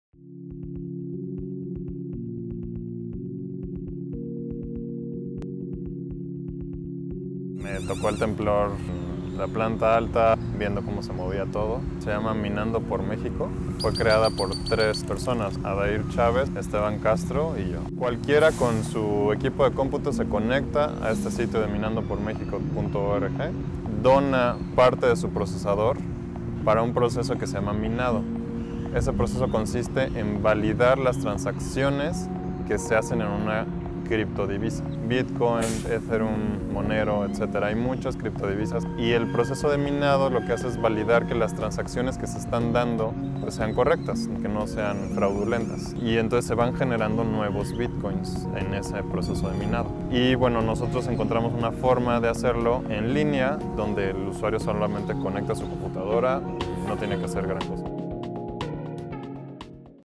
Iniciativas19: Minando por México
Iniciativas19 visibiliza proyectos de la sociedad civil durante y después del 19S.También es un fondo económico para consolidar acciones de reconstrucción a mediano y largo plazo. Iniciativas19 permite pensar en nuevos modelos de participación ciudadana que contribuyan a una CDMX más resiliente.